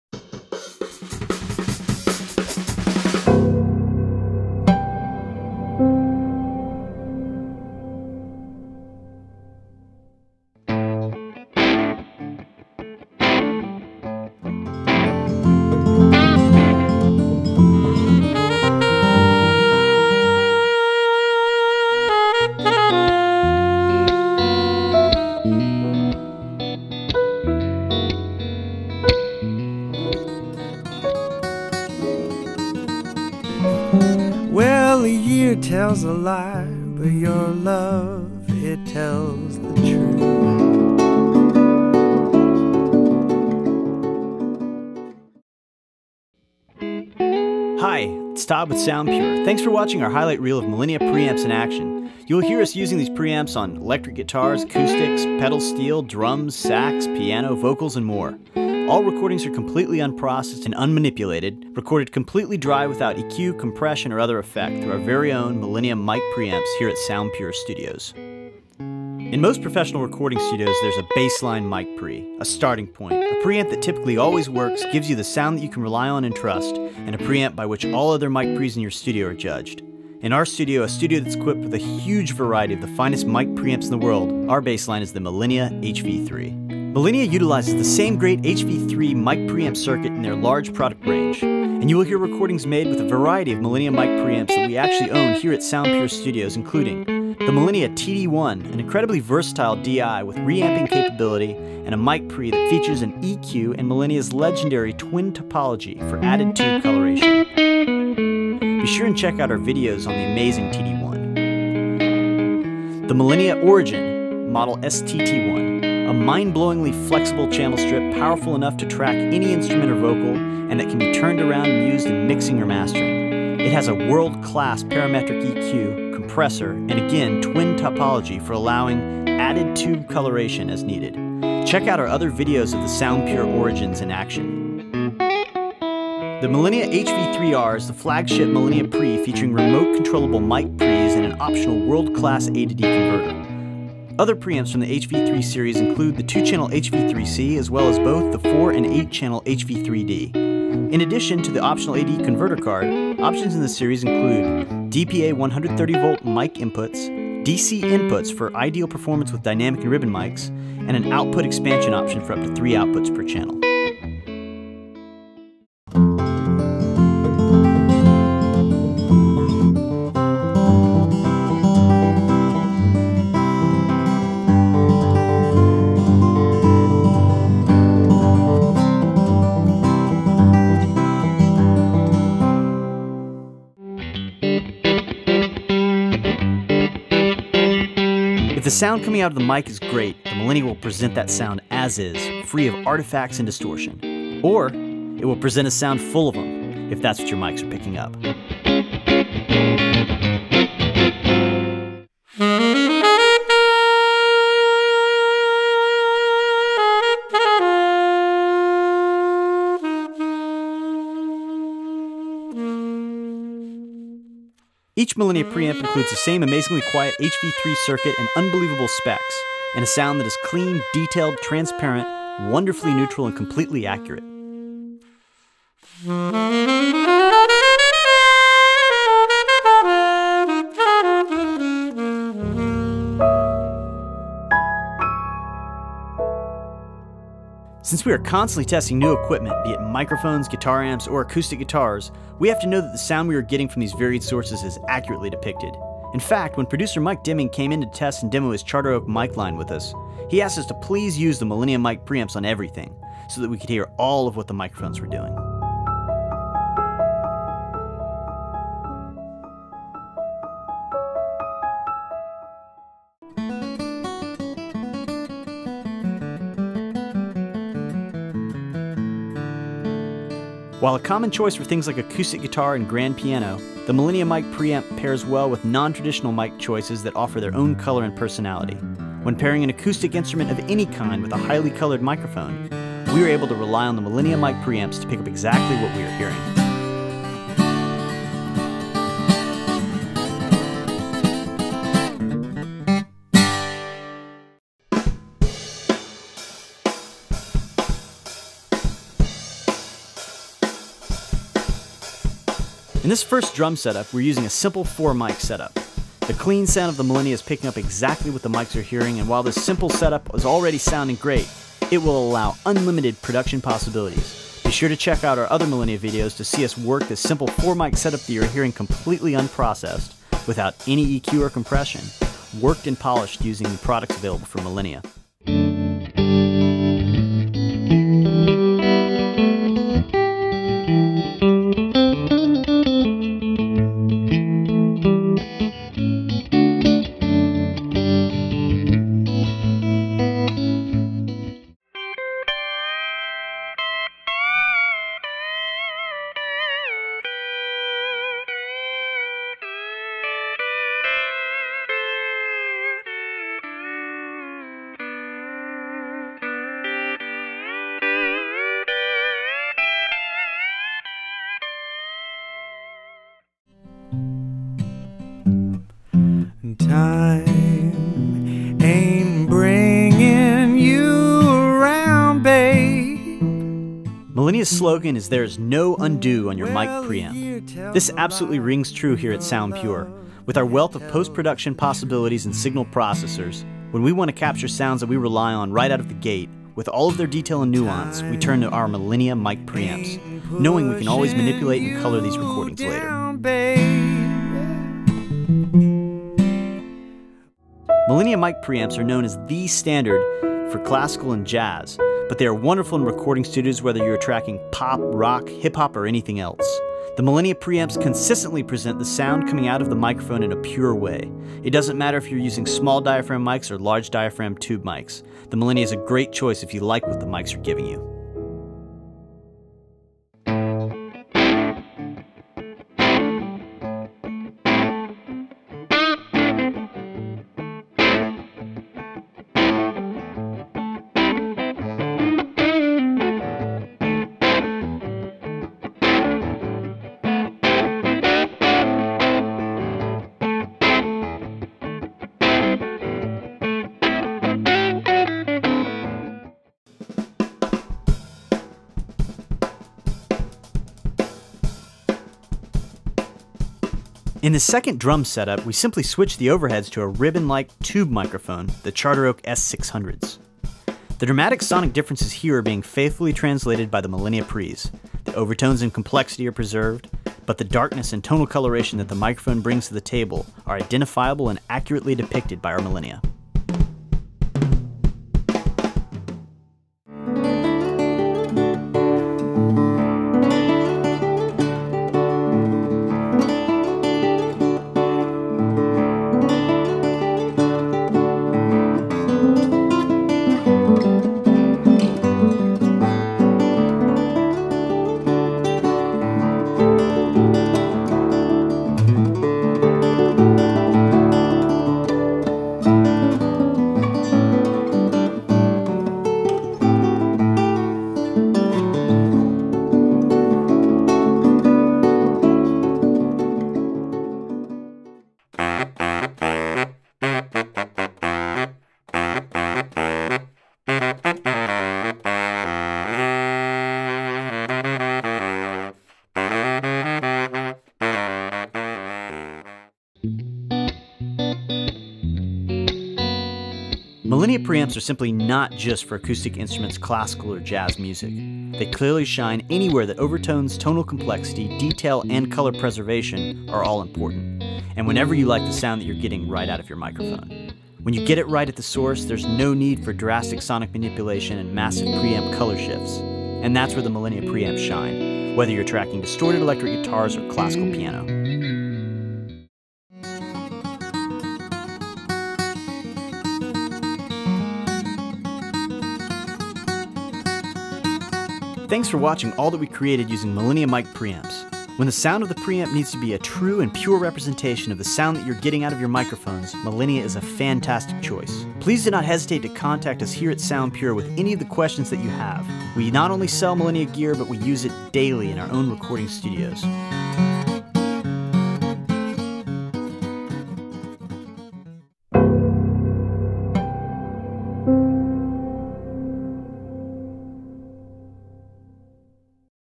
Millennia HV3 Mic Pre Video Demo & Highlight Reel
The Millennia HV3 is known as a world class microphone preamplifier.  The same great circuit design is used in a variety of Millennia preamps including the HV-3d (4 and 8 channell versions), the HV3c, the HV3r Remote Controllable Mic Preamp with optional A/D conversion card, the Millennia STT1 Origin Channel Strip, and the TD-1 DI and Mic Pre. While Millennia Mic Preamps are definitive in the classical and jazz worlds, it is truly outstanding in all serious recording studio applications, where the sound company out of the microphone is just right, as-is.